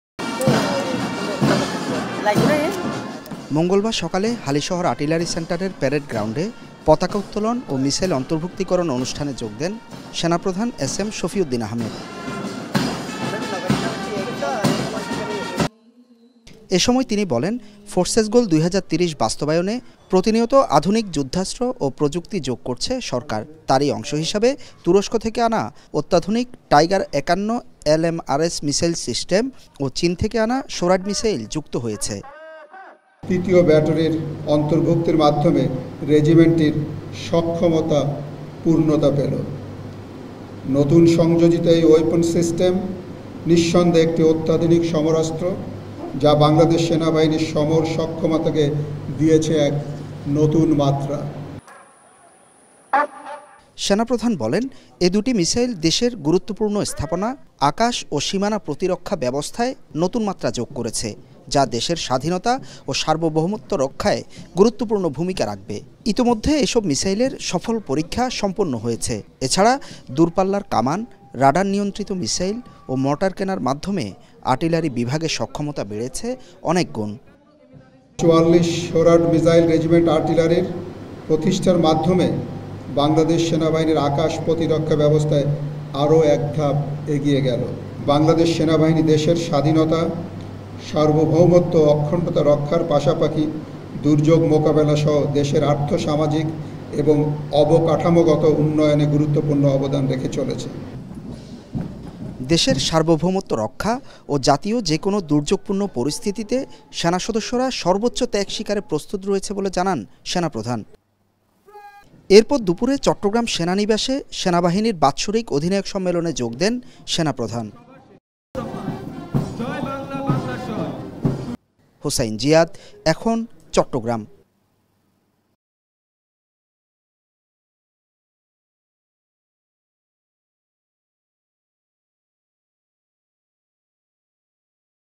বহরে যুক্ত হলো চীন-তুরস্কের অত্যাধুনিক মিসাইল | Bangladesh Army Missile System | Ekhon TV
বাংলাদেশ সেনাবাহিনীতে যুক্ত হলো তুরস্ক থেকে আনা অত্যাধুনিক ৫১ এলএমআরএস মিসাইল ও চীন থেকে আনা শোরাড মিসাইল। সকালে চট্টগ্রাম অর্টিলারি সেন্টারে দূরপাল্লার ক্ষেপণাস্ত্র দুটি উদ্বোধন করেন সেনাপ্রধান জেনারেল এস এম শফিউদ্দিন আহমেদ। এসময় তিনি বলেন, মিসাইল দুটি দেশের গুরুত্বপূর্ণ স্থাপনা ও আকাশসীমা রক্ষায় নতুন মাত্রা যোগ করেছে।

Our Office Address:
EKHON TV
City Park Lane
19 Hatkhola Road, Wari
Dhaka-1203
Bangladesh